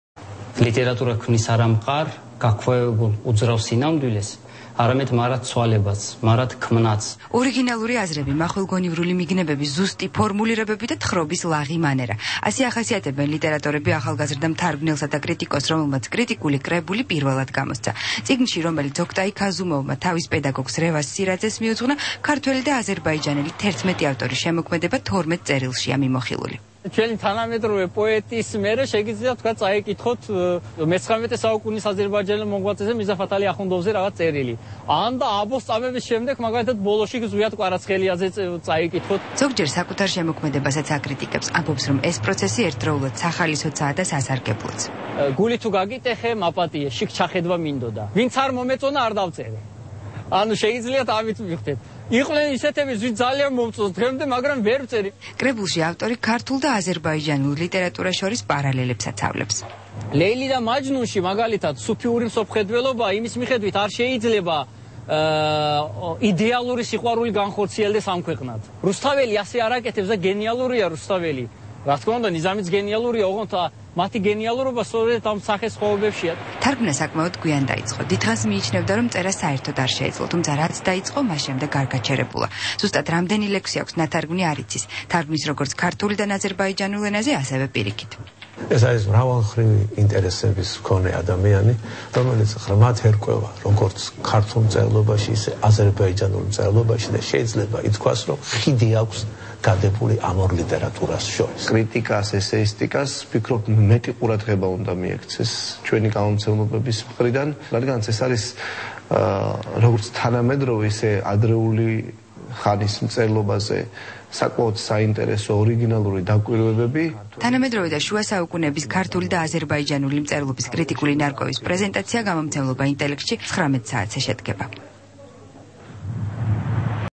"129 გვერდი ლიტერატურაზე"
გამომცემლობა "ინტელექტმა" ოქტაი ქაზუმოვის წიგნი "129 გვერდი ლიტერატურაზე" გამოსცა